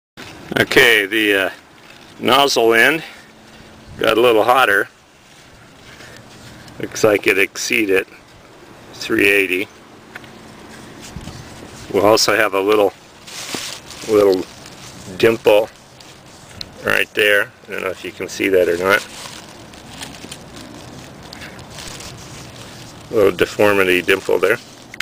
phenolic pt2
Sugar Shot to Space Program - examing the phenolic tube after removal from the Blast Tube Motor (part 2 of 2).